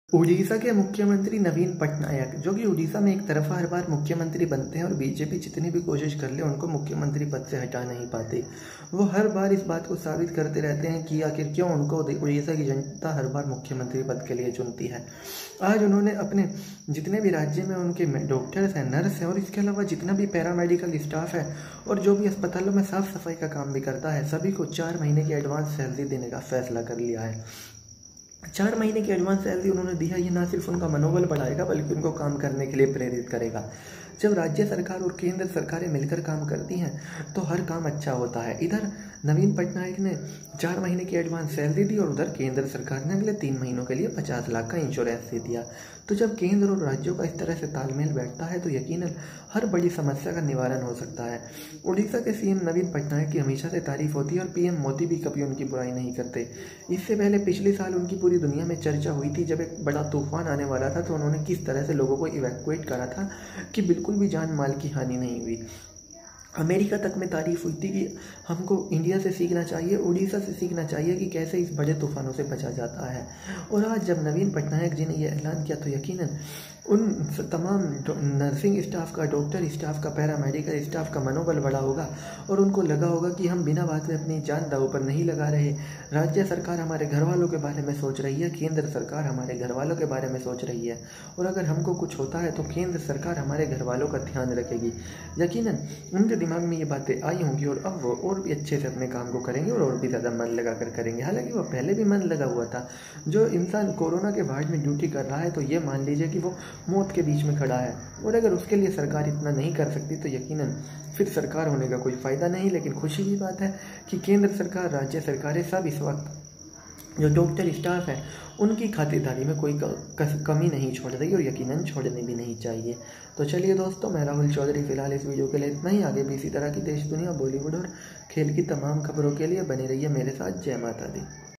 Naveen patnayak ने तो orrisa के paramedical staff को 4 months तक की salary पहले ही दे दी
Naveen patnayak paid advance 4 month salary to all the paramedical staff working in orrisa.